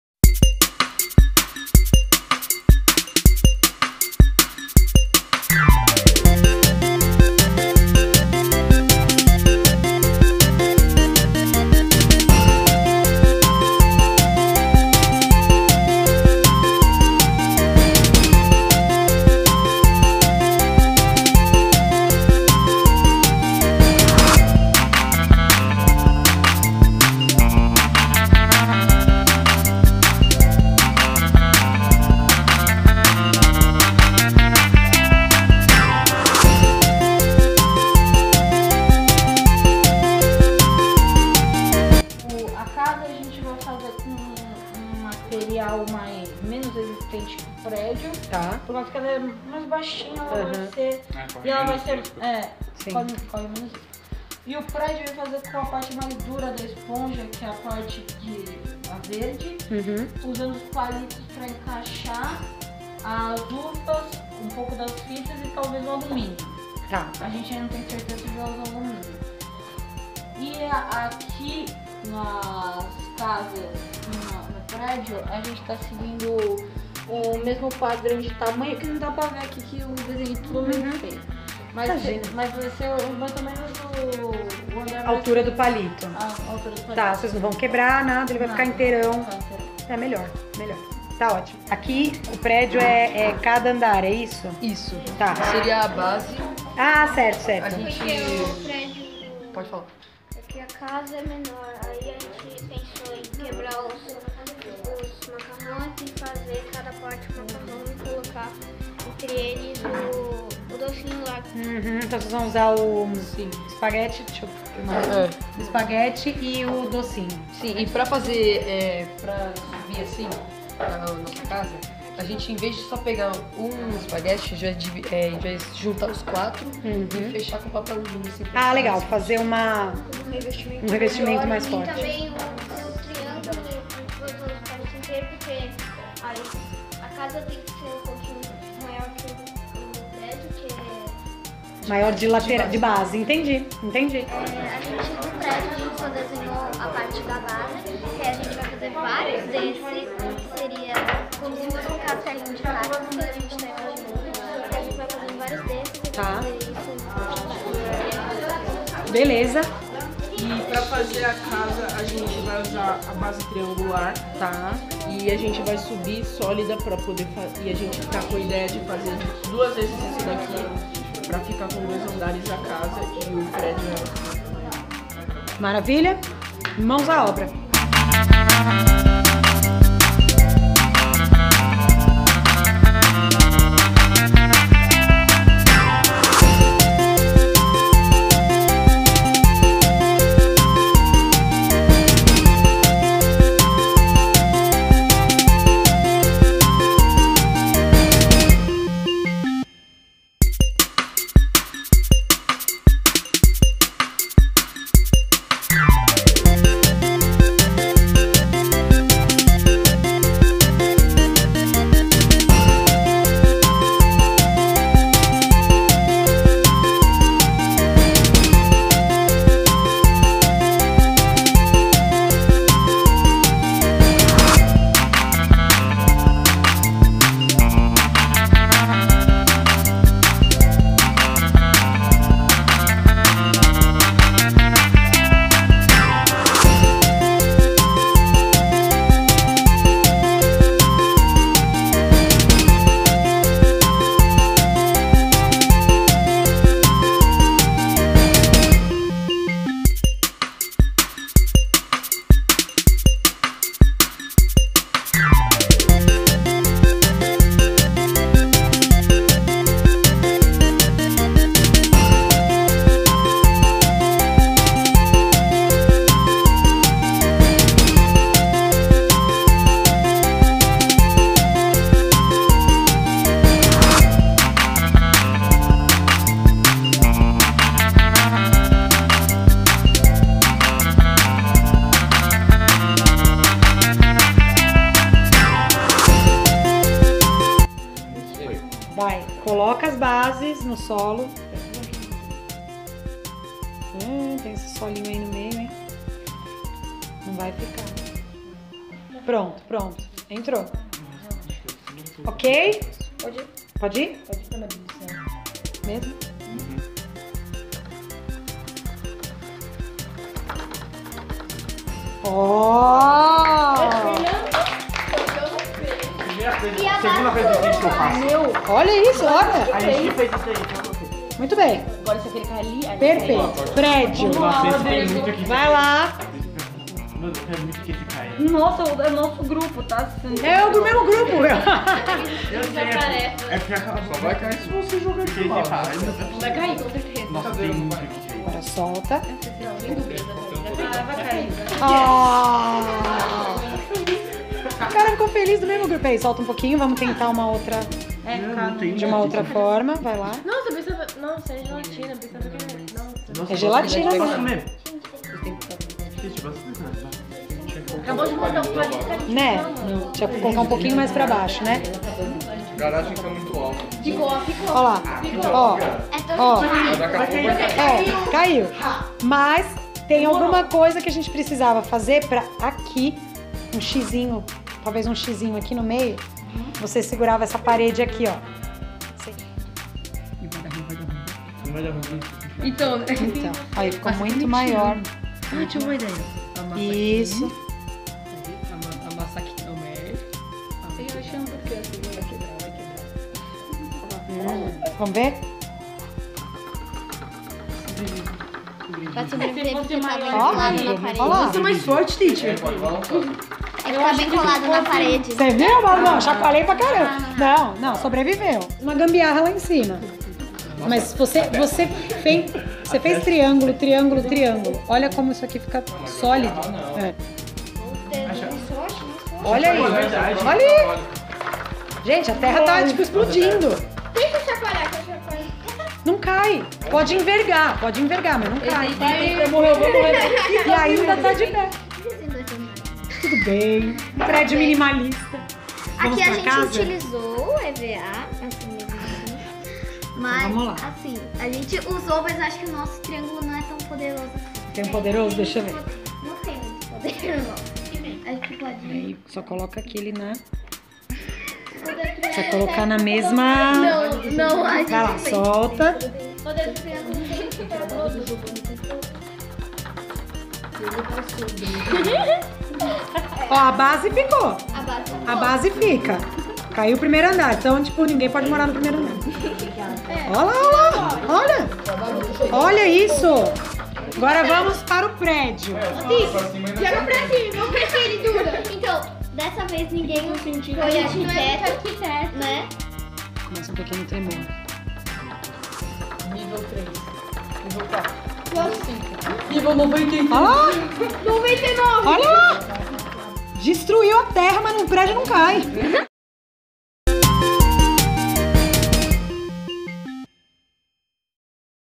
Curso STEAM: Aula 06 - A Problemática da Terra Instável (O Terremoto)
A cidade de Poço Profundo sofreu um terrível terremoto na semana passada. Mas nossos alunos voltaram no tempo para ajudar a cidade construindo prédios resistente. Será que eles conseguiram evitar o pior? Assista essa aventura!